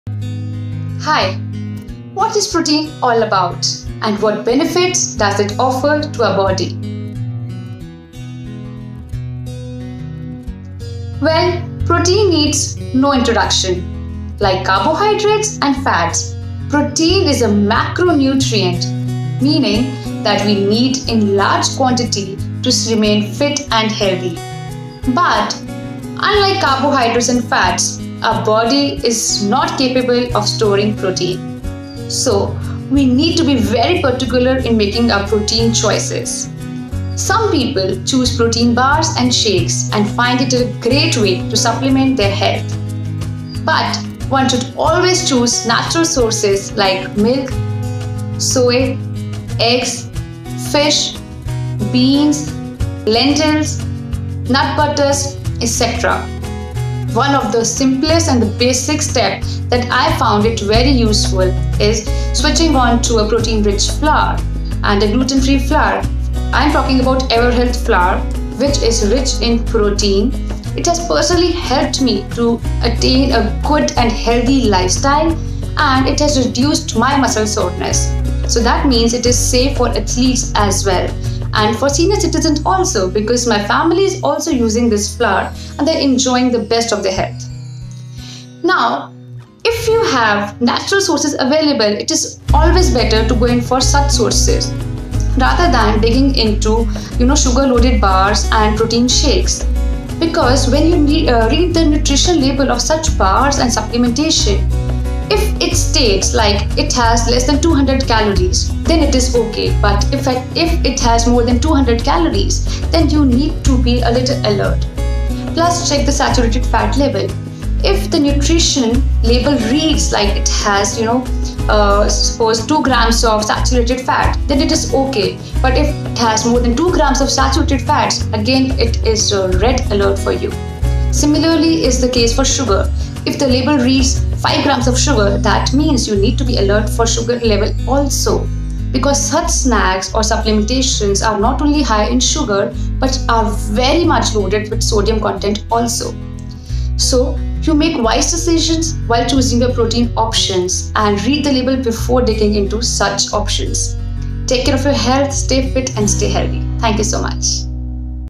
PROTEIN- An Underestimated “macronutrient”
Let’s learn about the macronutrient, Protein, that is underestimated but is loaded with health benefits.

Thanks to the Shveta Mahajan, Nutritionist & Diet Consultant, for enlightening us.